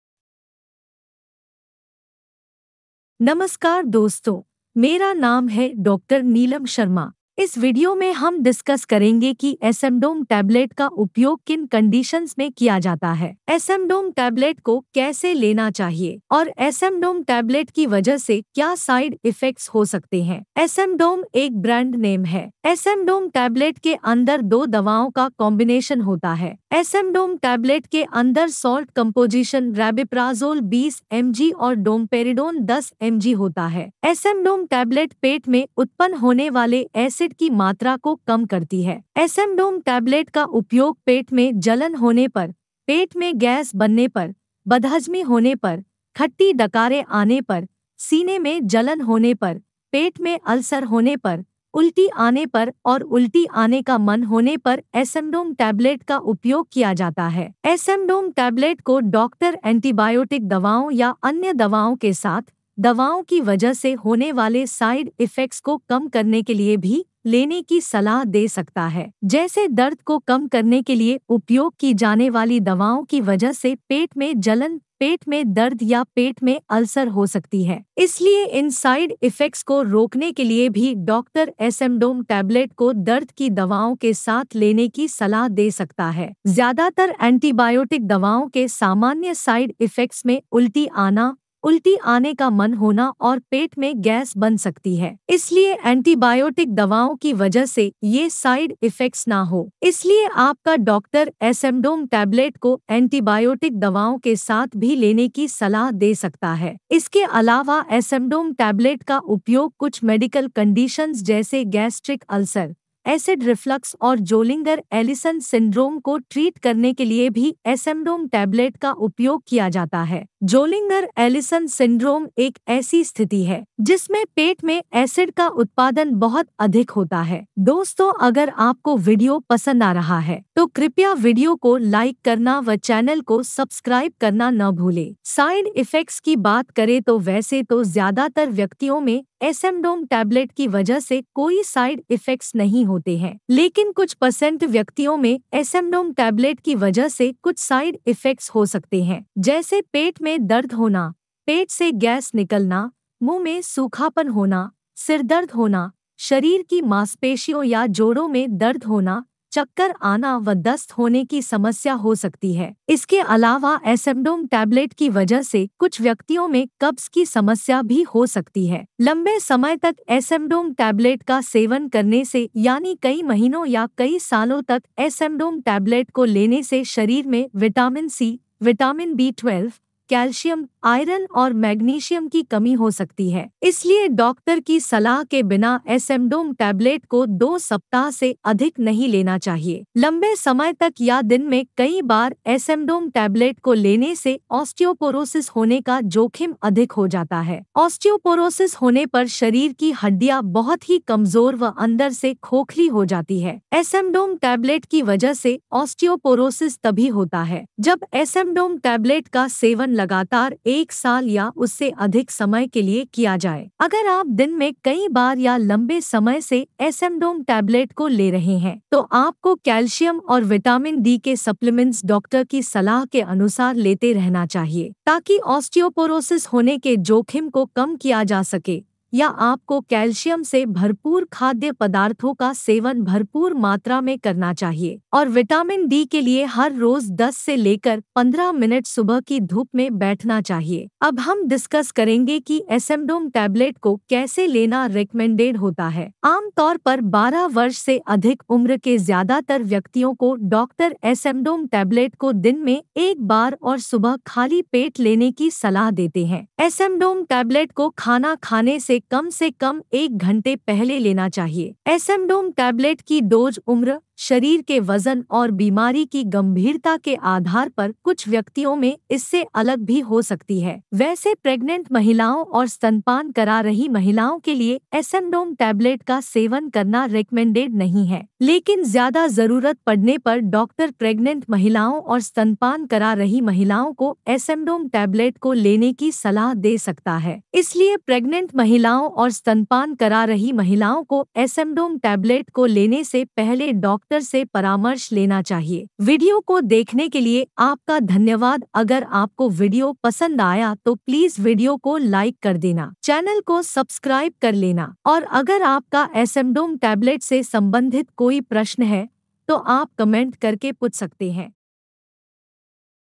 Smdom Tablet
smdom tablet uses & side effects | smdom tablet ke fayde aur nuksan | how to use smdom tablet. in this video, we will discuss what are the uses and benefits of smdom tablet. How to use smdom tablet. We will also discuss what are the side effects of smdom tablet. Watch the complete video for detailed review of smdom tablet in hindi.